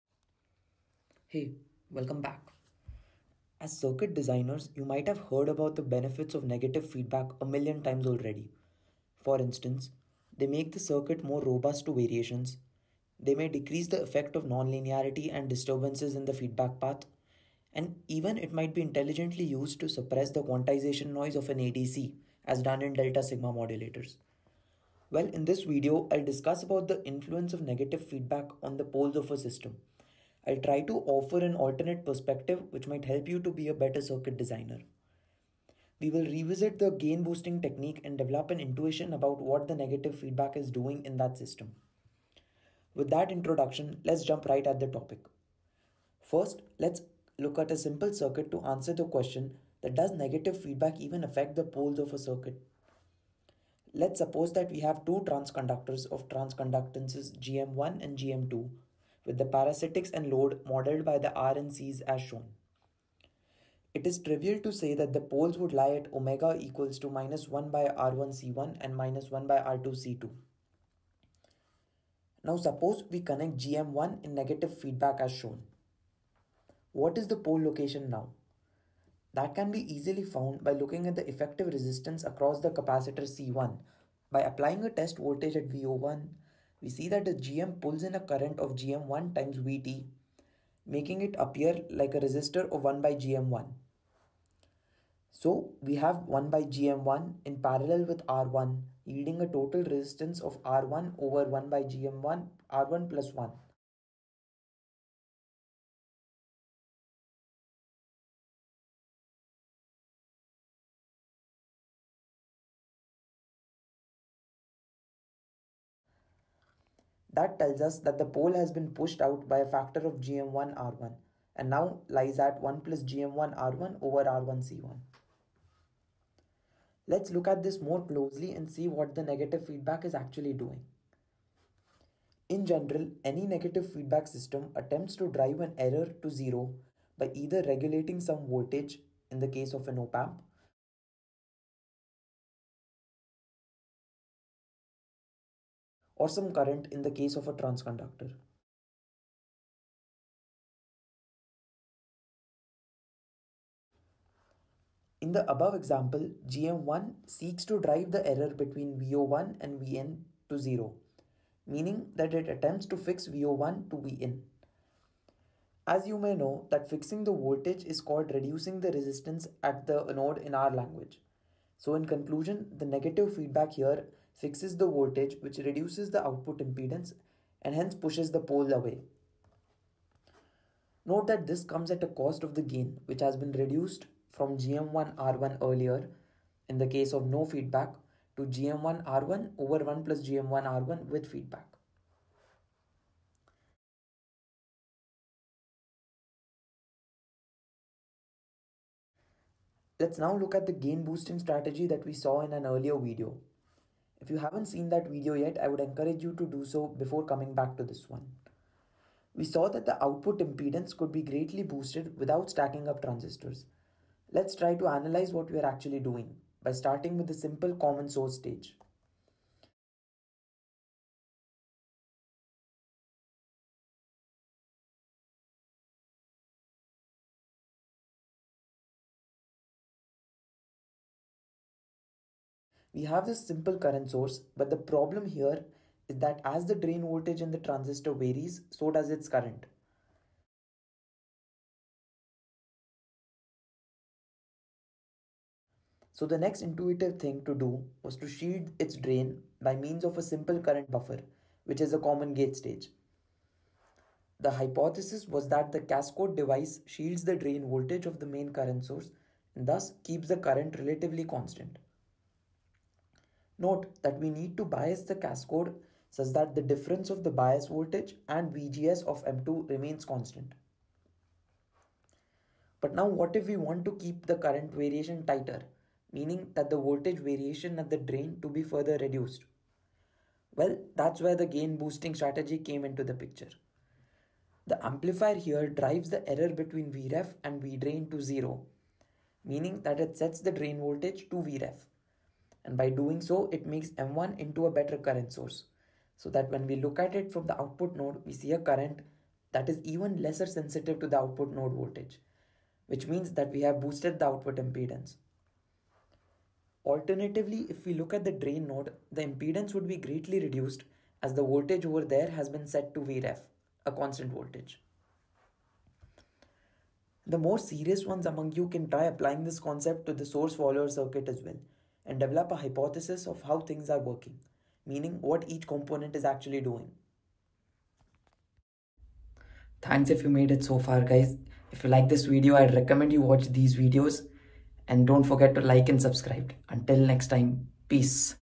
Influence of Negative Feedback on the poles of a circuit
In the following video, I discuss about the influence of -ve feedback on the poles of a system. I try to offer an alternate perspective which perhaps might sound intriguing.

We will revisit the gain-boosting technique and develop an intuition about what the negative feedback is doing in that system.

More specifically, we will analyse the basic premise of a negative feedback system and how that can help us to increase/decrease the input impedance of a node, thereby manipulating the pole locations.